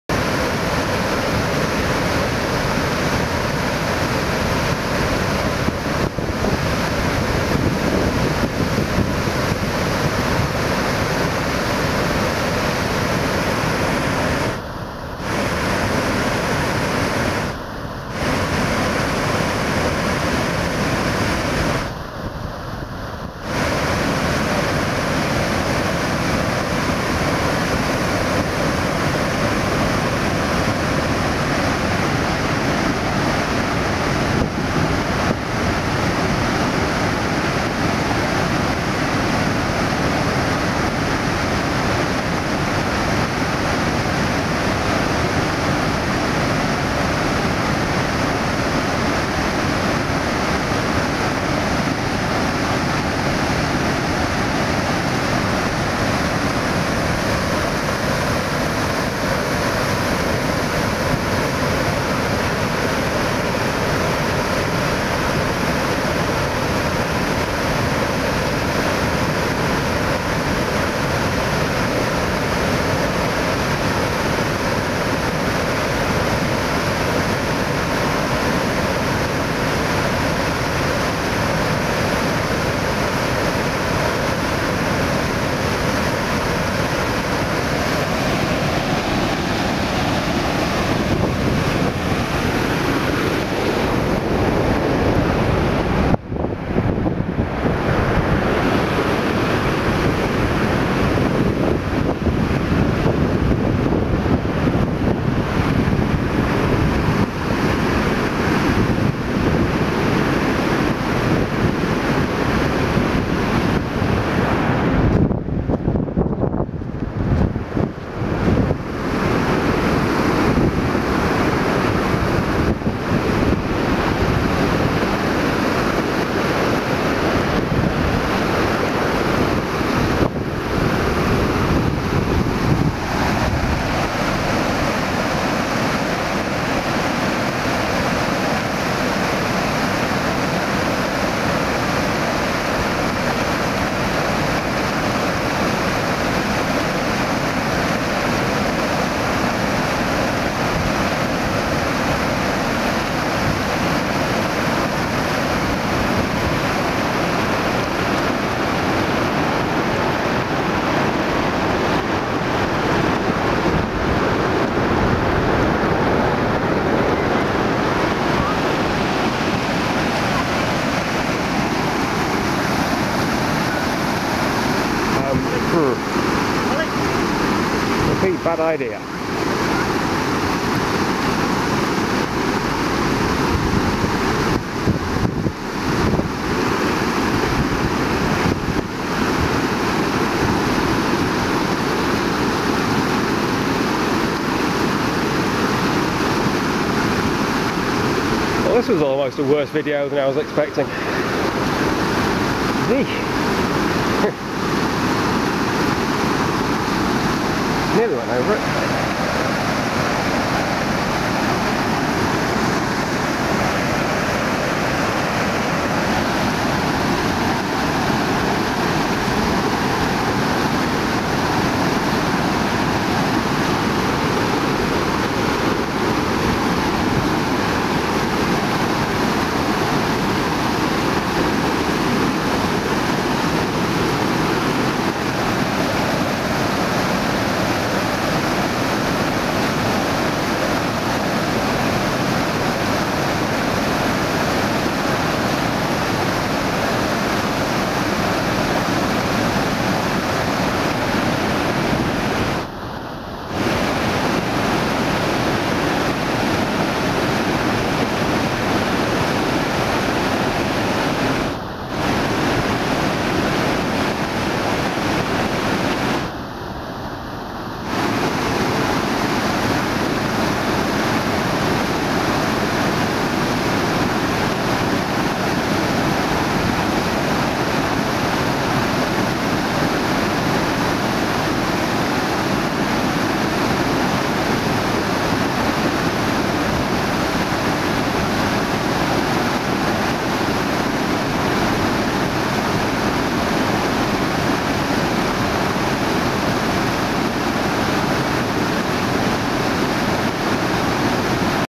Swollen River Ure at Aysgarth 02 Oct 2019
Aysgarth Upper Falls with a lot of water going over them after recent heavy rain.